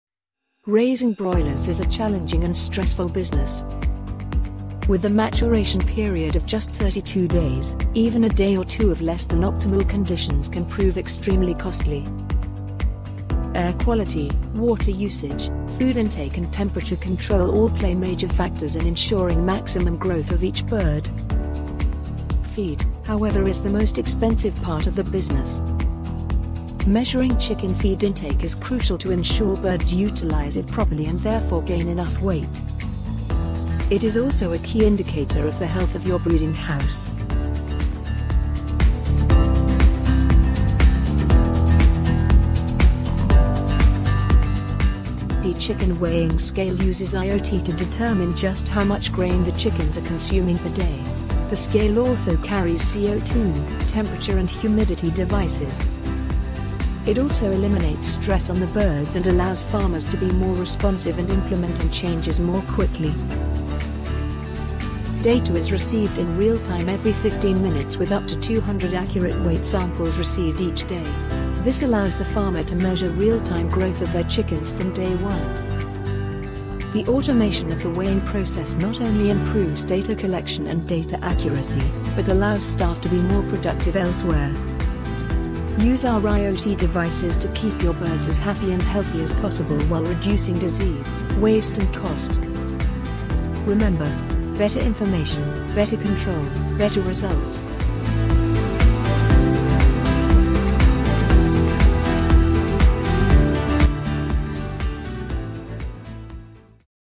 ARCC Agri IOT Chicken Weighing Scale
Air quality, water usage, food intake and temperature control are all major factors in ensuring maximum growth of each bird. Measuring feed intake is crucial to ensure birds are using feed properly and gaining enough weight. Our ARCC Chicken Weighing Scale uses IoT to determine just how much grain is being consumed per day. The scale also carries co2, temperature and humidity devices. With live readings taken every 15 minutes and up to 100 readings per day, farmers are provided with live up to date information whenever they need it.